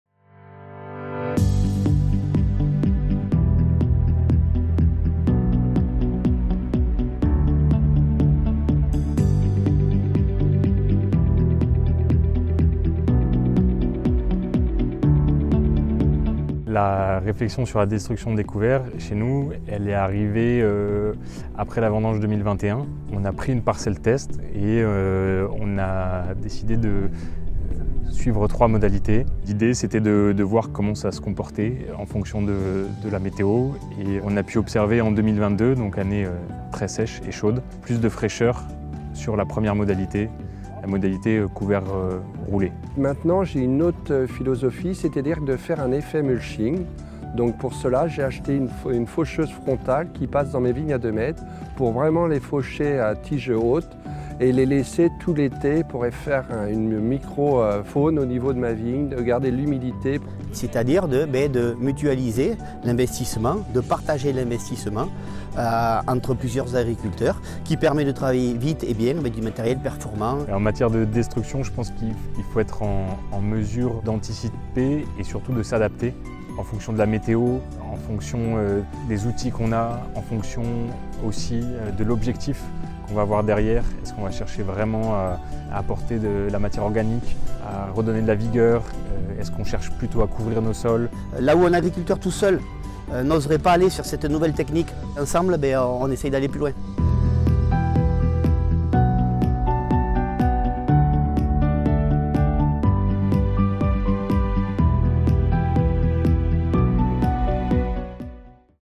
Arrêt sur images - Rallye Transfert NA / Couverts végétaux : une destruction à adapter
Ce clip "Arrêt sur images" propose des extraits des vidéos Rallye Transfert Nouvelle-Aquitaine, à retrouver dans leur intégralité sur la playlist